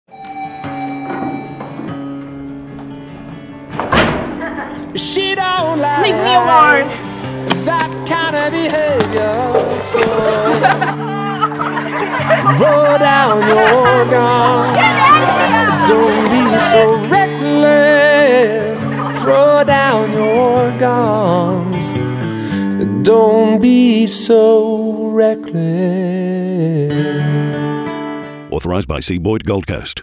STOP BULLYING NOW - Part Of The Crowd
Stop Bullying Now 2018 - Part Of The Crowd
Focusing on the bystanders to bullying situations and uniquely shot from a first person perspective witnessing a bullying event in person and then watching back the recorded event online. This hard-hitting commercial proves thought on bystanders and their roles in bullying.

Co-produced with Tafe Screen & Media students

Jack Boulter-Milne - Writer Director
Jordan Skuse - Director Of Photography.
Tyler Chamberlain- Producer/Post Production
Melinda Rossouw-Make-up/Production Deisgn

Executive Producer - Christopher Boyd
Technical Director and Producer - Thomas Smith

TAFE QLD - Jed Cahill, Karen Graham, Richard Barrett.